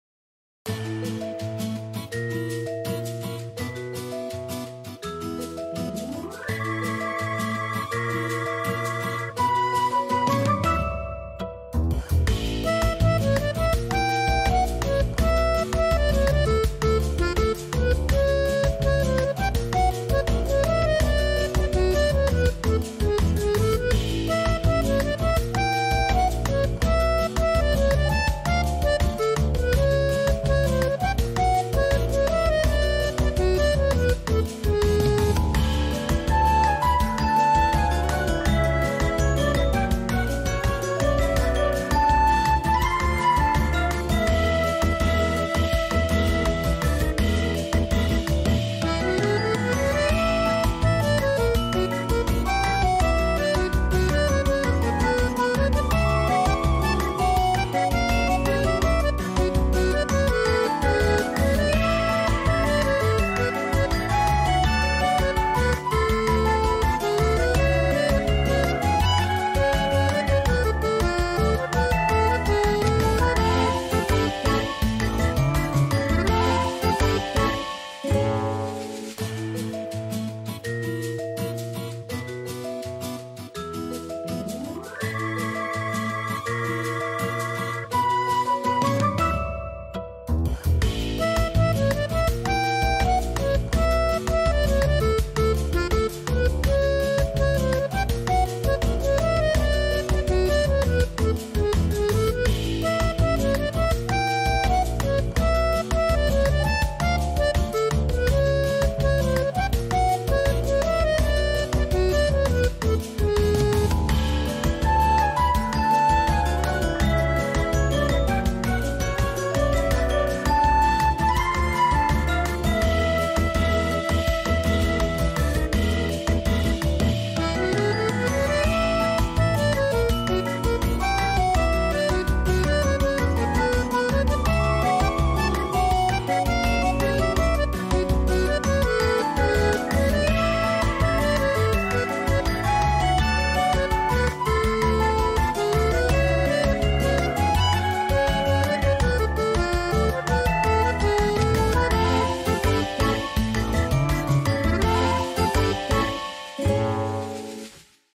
妻と仲良く朝ごはん（これまた久しぶりに「オムナポリ」でーす♪）【朝活チャレンジ598】
本日は、久しぶりに「オムナポリ」です！
毎回毎回同じ芸風でスミマセン…カミさんのリクエストです(^_^;)